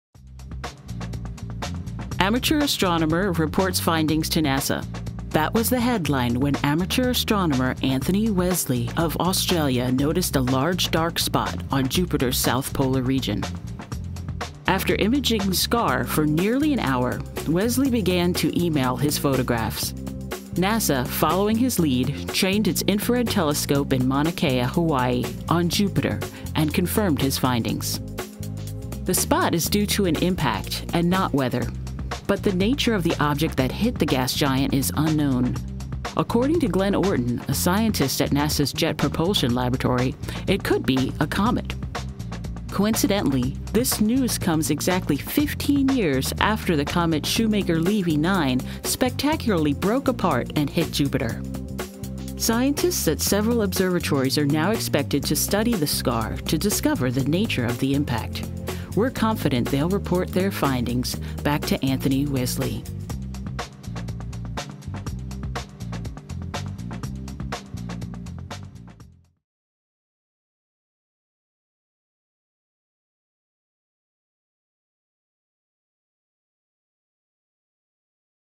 Science Today: Jupiter's New Spot | California Academy of Sciences
An amateur astronomer discovers a new, mysterious spot on Jupiter.